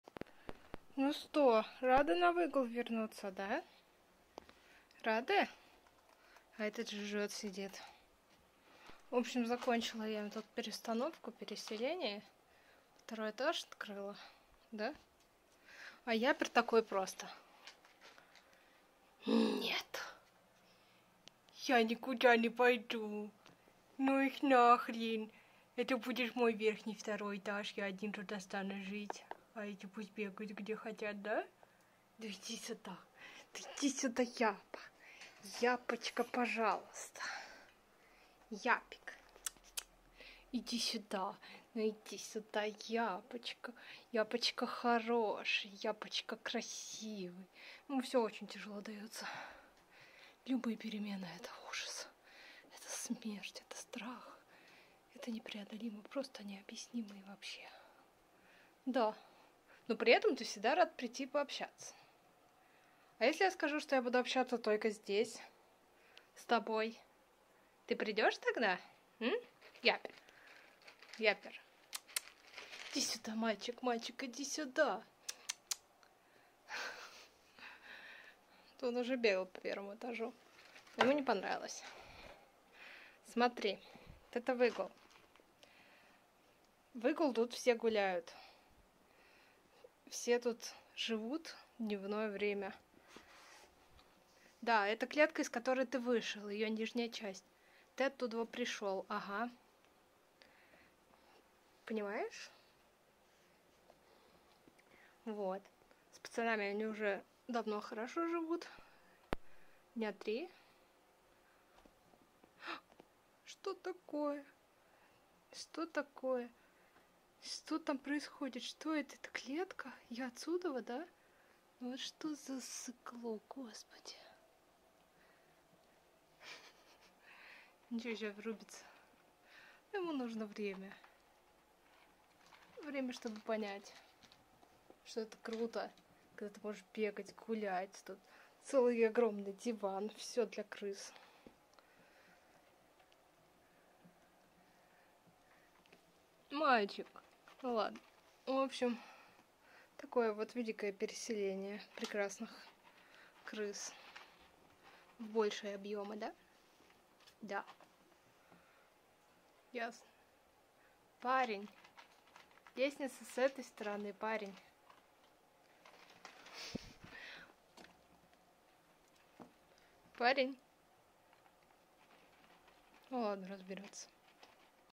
Япер потерялся в этой жизни, а альбиносы счастливы
Япер впервые в жизни на этом выгуле, а вот альбиносы просто рады на него вернуться.